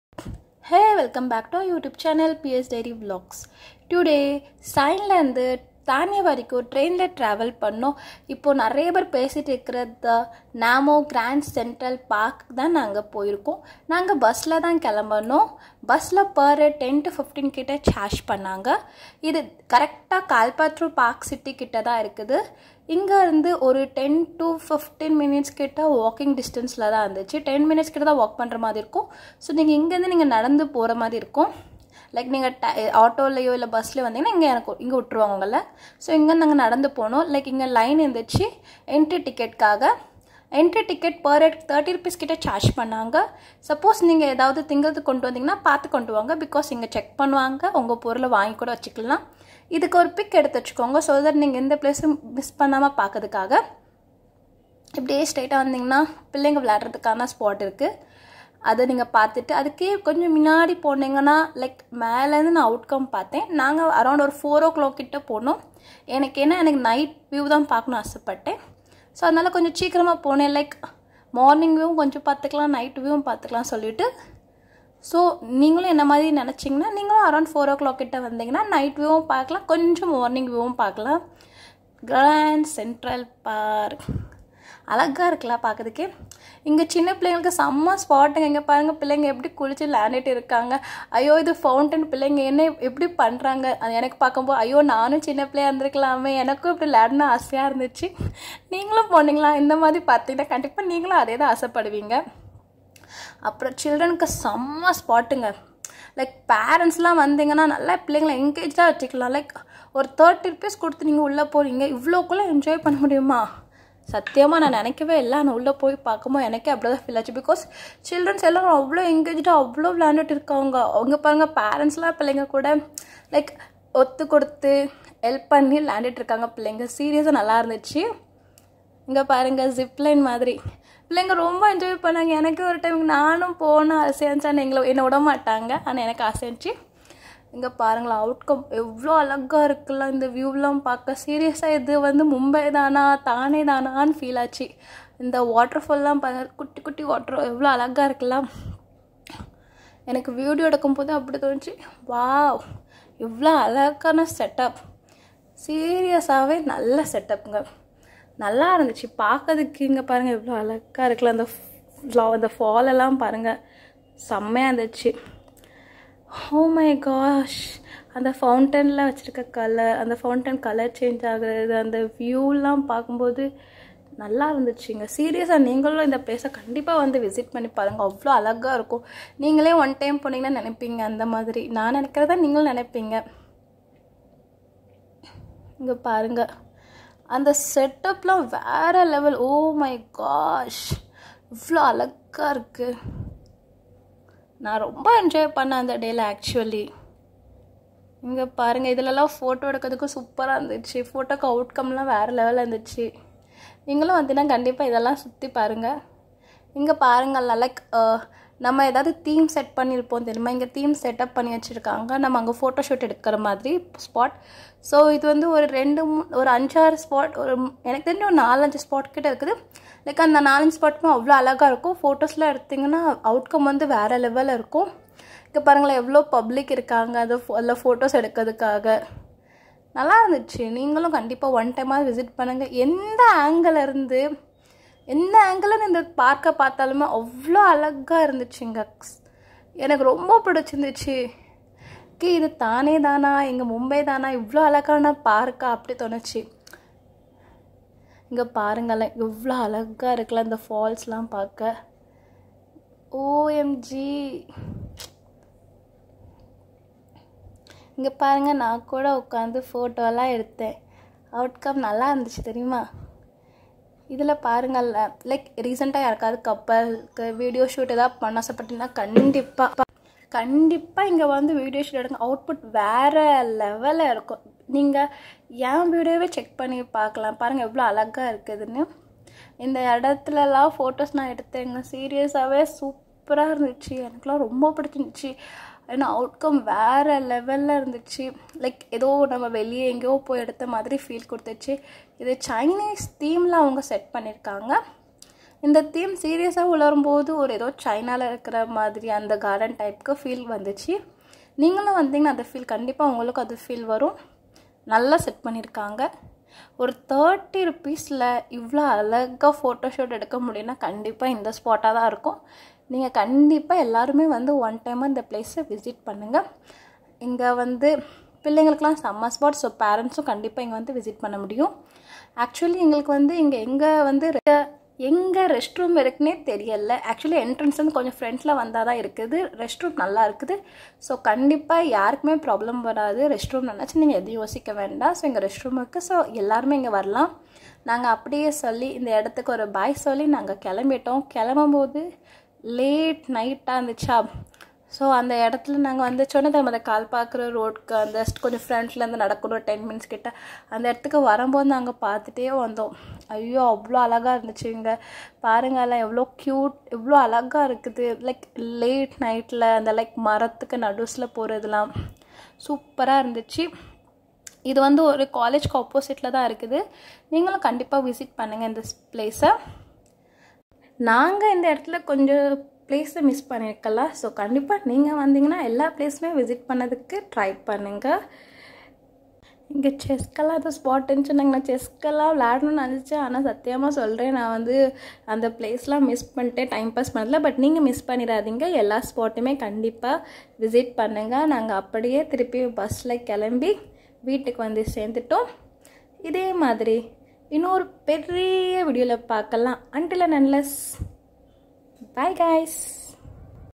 NaMo Grand Central Park for 1 Day Picnic Spot
Namo Grand Central Park Kalptaru ParkCity Thane Mumbai

Sion - Thane Train Ticket  Was 20Rs return

Bus From Thane TO Kalptaru Parkcity was 18Rs if  it was Ac otherwise 15Rs

Park is Open From Tuesday to Sunday it has been closed on Monday

Timing to Visit the Park :
Morning  : 6am to 11am
Evening : 4pm to 9pm

Entry Ticket charge When It was weekdays :
for adults : 20Rs
for senior citizen  : 15Rs
for kids : free

Entry Ticket  charge when it was weekends  :
for adults : 30Rs
for kids : free